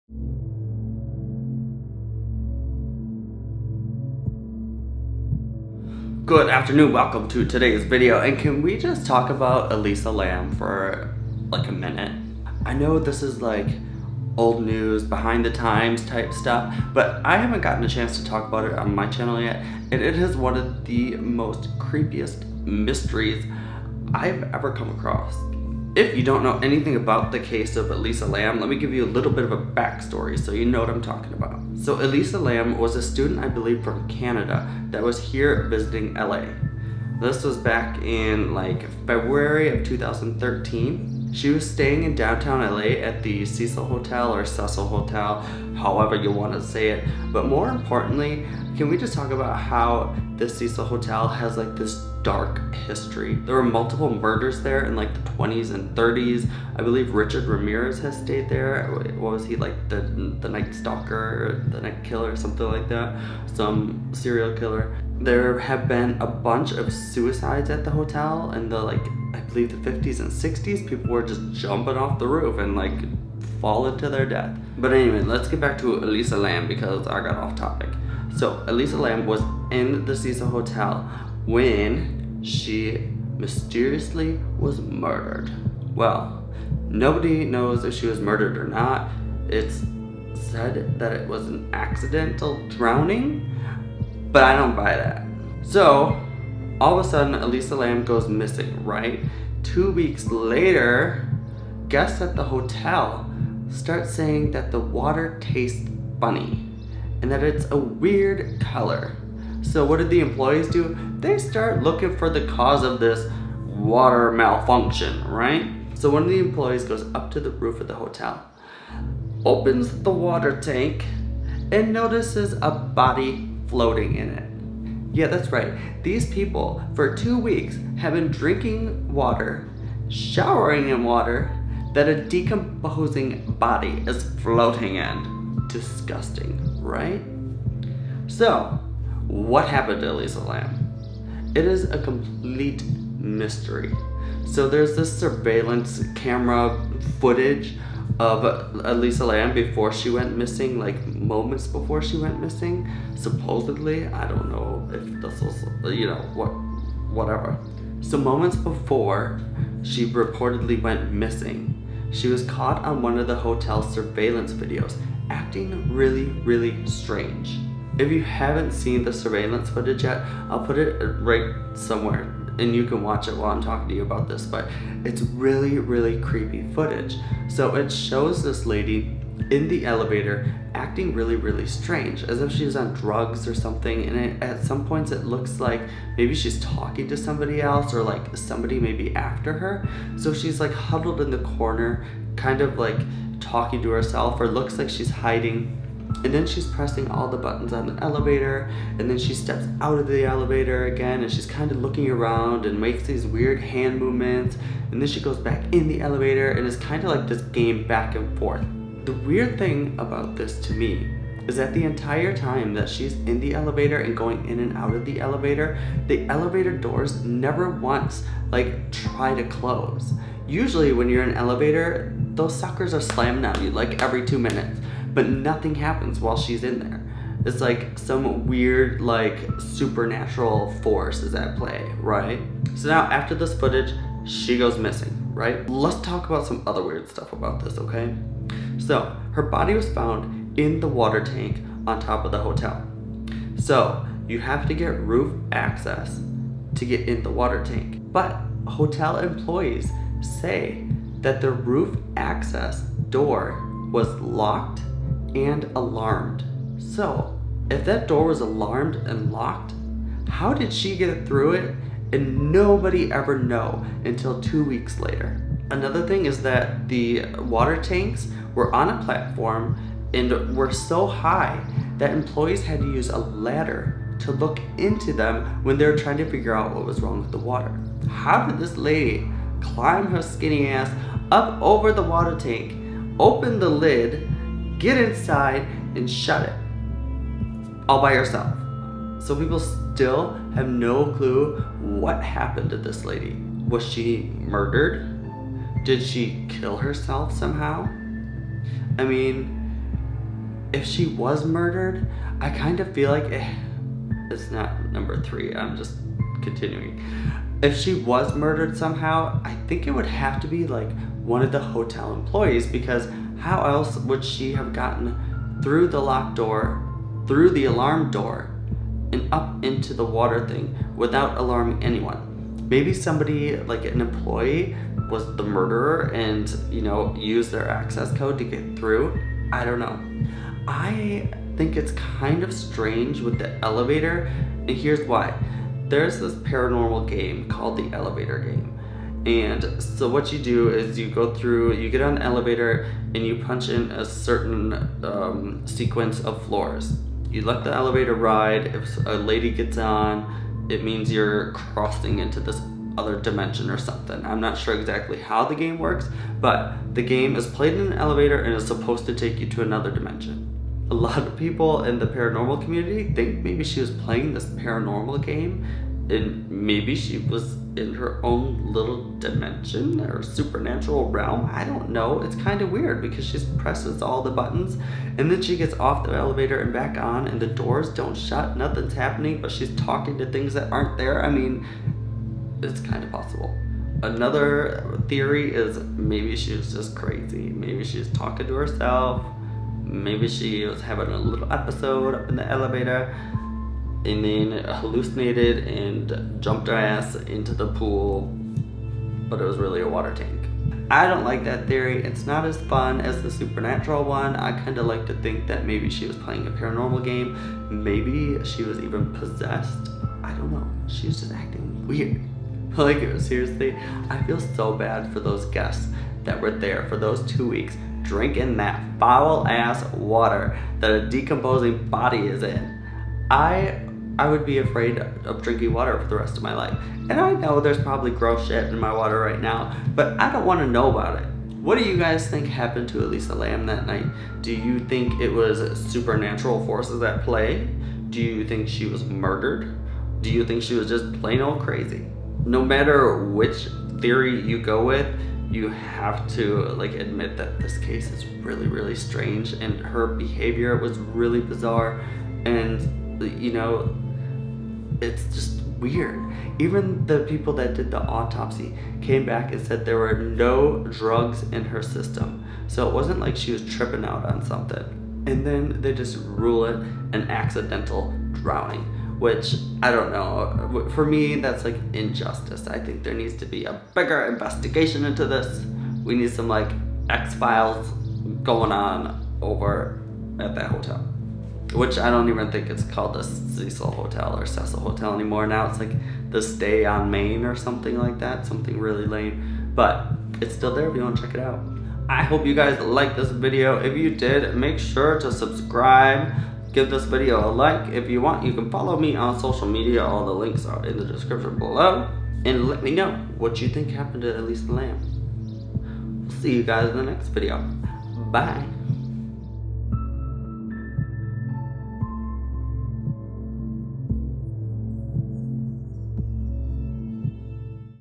Elisa Lam Mystery | MichaelScot
Welcome,

Today's video is about the weird and mysterious circumstances revolving around the story of Elisa Lam.

What do you think happened to Elisa Lam?

Music